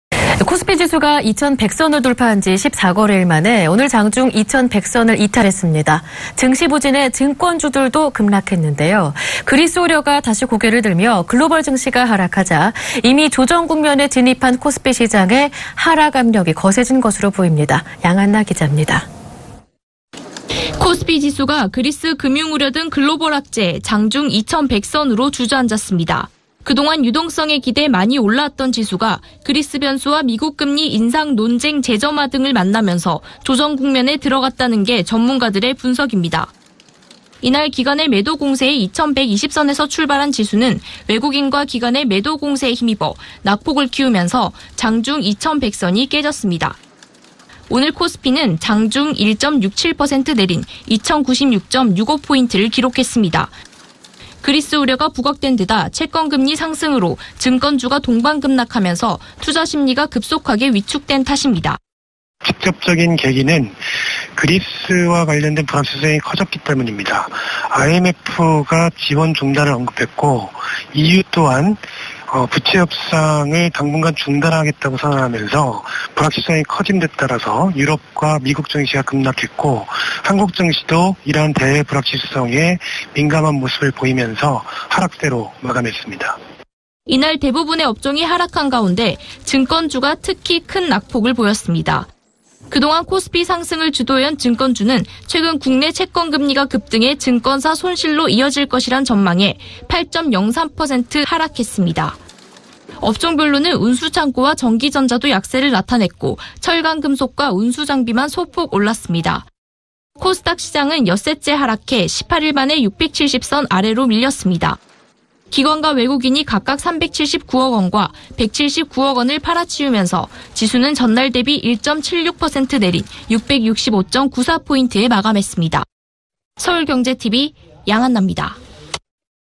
코스피 장중 2,100선 깨져 증권주 8%↓
[앵커]
코스피지수가 2,100선을 돌파한지 14거래일만에 장중 2,100선을 내줬습니다. 증시 부진에 증권주들도 급락했습니다.
그리스 우려가 고개를 들며 글로벌 증시가 하락하자, 이미 조정국면에 진입한 코스피 시장에 하락 압력이 거세진 것으로 보입니다. 양한나기자가 전합니다.

[기자]
코스피지수가 그리스 금융 우려 등 글로벌 악재에 장중 2,100선으로 주저앉았습니다. 그동안 유동성에 기대 많이 올라왔던 지수가 그리스 변수와 미국 금리 인상 논쟁 재점화 등을 만나 조정 국면에 들어갔다는 게 전문가들의 분석입니다.
이날 기관의 매도 공세에 2,120선에서 출발한 지수는 외국인과 기관의 매도 공세에 힘입어 낙폭을 키워 장중 2,100선이 깨졌습니다. 오늘 코스피는 장중 한때 전거래일 대비 35.58포인트(1.67%) 내린 2096.65를 기록했습니다.
그리스 우려가 부각된데다 채권금리 상승으로 증권주가 동반 급락하면서 투자심리가 급속하게 위축된 탓입니다.

[인터뷰]
오승훈 팀장 / 대신증권
“직접적인 계기는 그리스와 관련된 불확실성이 커졌기 때문입니다. IMF가 지원 중단을 언급했고 EU 또한 부채 협상을 당분간 중단하겠다고 선언하면서 불확실성이 커진데 따라서 유럽과 미국 증시가 급락했고 한국 증시도 이런 대외 불확실성에 민감한 모습을 보이면서 하락세로 마감했습니다.”

이날 대부분의 업종이 하락한 가운데 증권(-8.03%)주의 낙폭이 특히 컸습니다. 그동안 코스피 상승을 주도해온 증권주는 최근 국내 채권금리가 급등해 증권사 손실로 이어질 것이란 전망에 8.03% 하락했습니다.
업종별로는 운수창고(-2.97%)와 전기전자(-2.62%)도 약세를 나타냈고 철강금속(0.86%)과 운수장비(0.04%)만 소폭 올랐습니다.

코스닥시장은 엿새째 하락하면서 18일 만에 670선 아래로 밀렸습니다. 기관과 외국인이 각각 379억원, 179억원을 팔아치우면서 지수는 전 거래일보다 11.96포인트(1.76%) 내린 665.94에 마감했습니다.

서울경제TV 양한나입니다.

[영상편집 김지현]